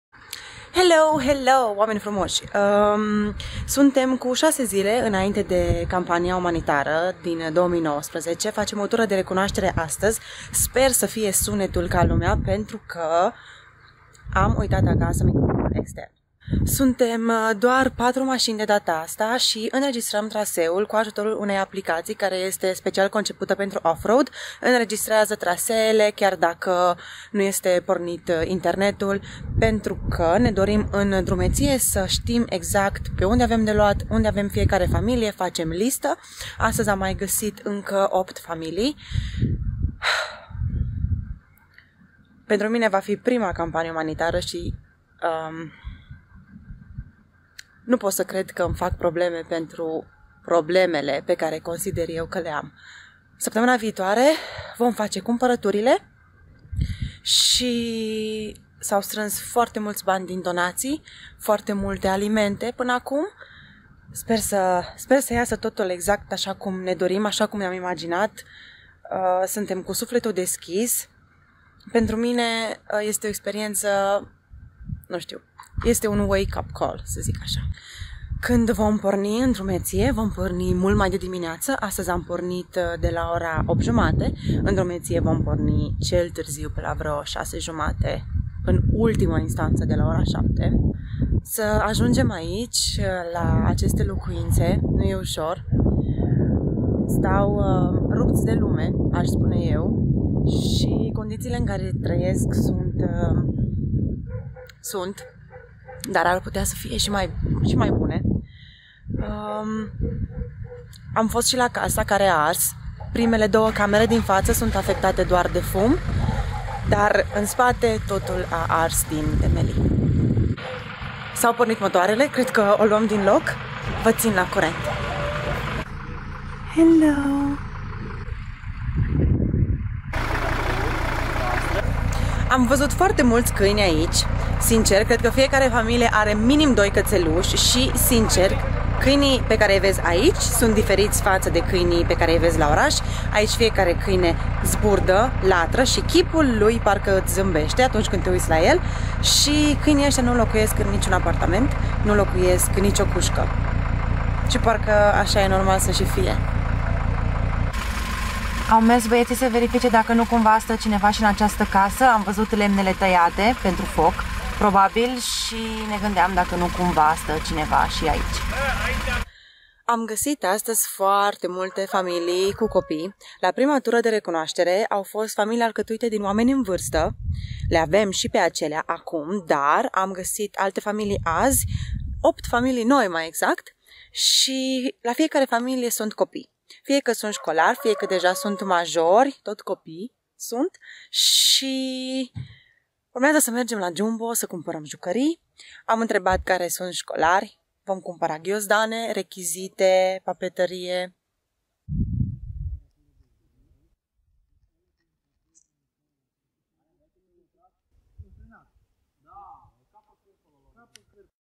Campanie Umanitara cu West Alpine Off Road - Iarna 2019 - Part 1.
CAMPANIA UMANITARA WEST ALPINE OFF ROAD 2019

PARTEA 1

Veniti cu mine in campania umanitara de anul acesta!
Hai sa facem fapte bune, impreuna.

Love, Daiana
The girl with the red lips.